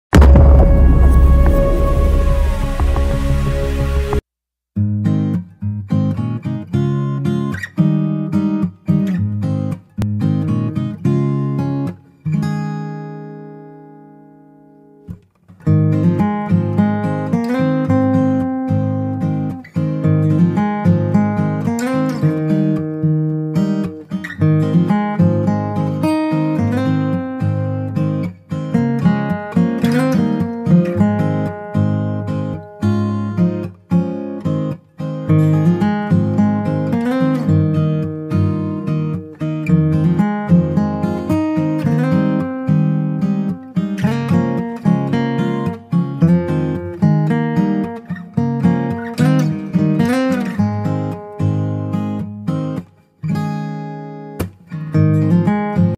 NIUPIKA Accendino accenditore torcia automatica per saldatura a Gas saldatura batteria portatile
Link to the same product:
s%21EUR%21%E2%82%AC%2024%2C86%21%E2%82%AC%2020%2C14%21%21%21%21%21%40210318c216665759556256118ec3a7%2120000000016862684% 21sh

Contact customer service can get a gift, welcome to your presence.

Descrizione：
Uns off 2 batterie AA (le batterie non sono incluse)
Accendino Micro torcia, si siede sul tuo banco da lavoro senza occupa molto spazio
Puo utilizzare l'accenditore con entrambe le torce propano, acetilene, ossigeno (adatto a qualsiasi gas combustibile)
Istruzioni per l'uso: c 'e un piccolo pulsante/interruttore argento sull'accenditore che si trova in un canale a forma di U. Quando lo premi con la tua torcia con la punta della tua torcia appena leggermente oltre l'interruttore le scintille dell'accenditore e illumina la tua torcia